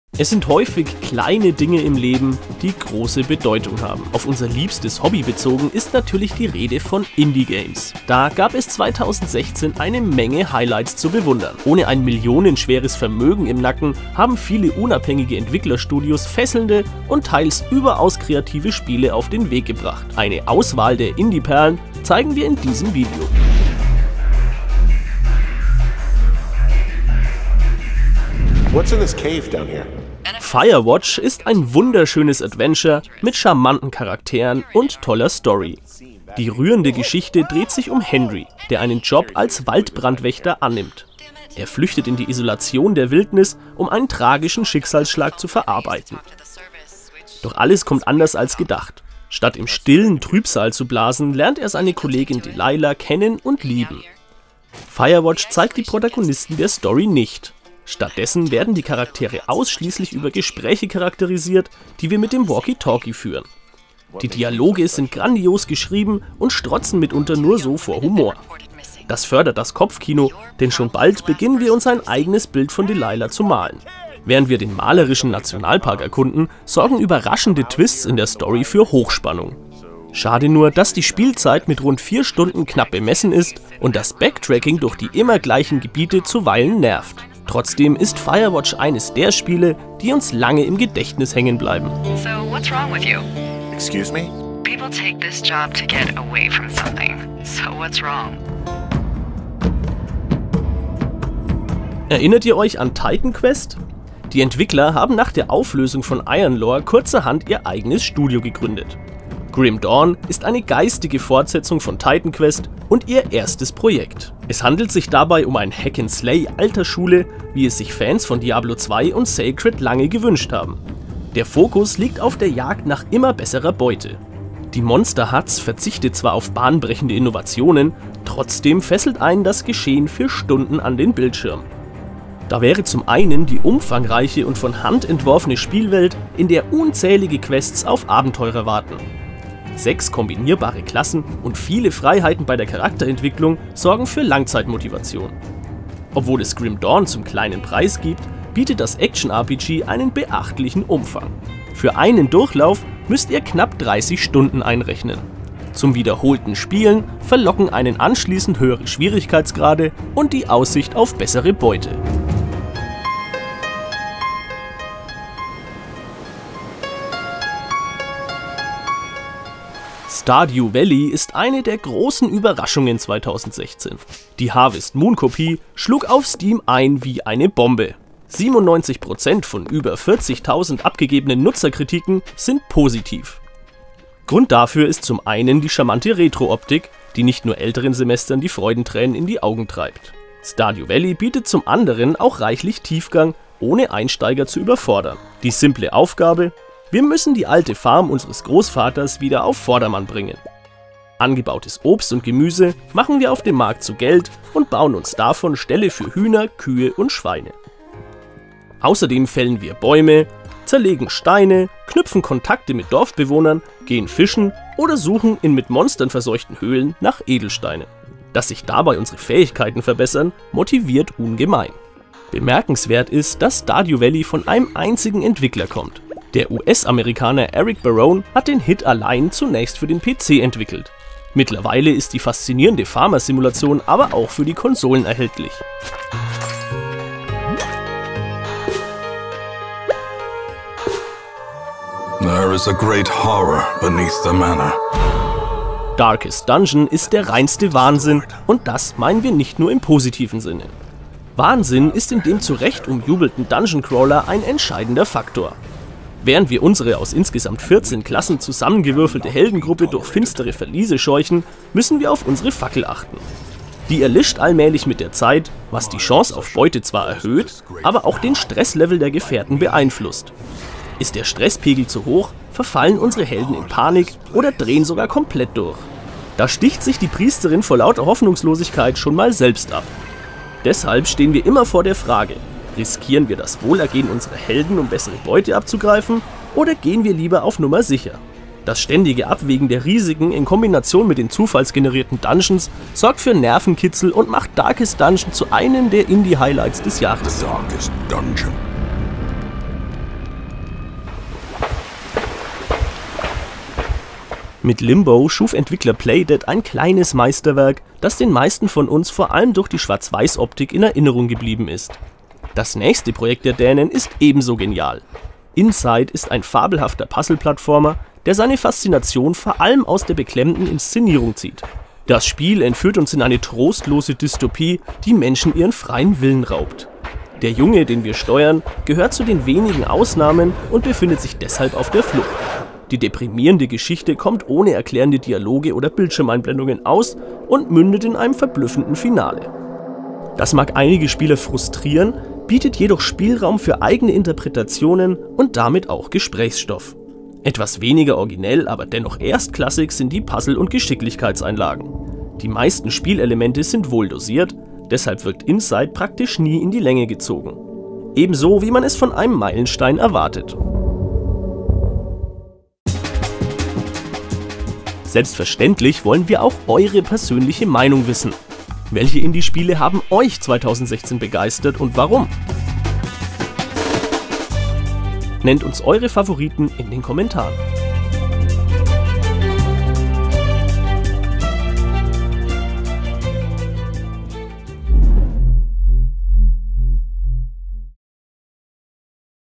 Indie-Hits 2016 im Rückblick - Von Firewatch bis Stardew Valley
Es braucht keine teuren Budgets, um tolle Spiele zu produzieren. Diesen Beweis traten 2016 viele Indie-Studios an. Kleinere Produktionen wie Firewatch und Stardew Valley haben uns nachhaltig beeindruckt. Warum, zeigt unser Video-Rückblick!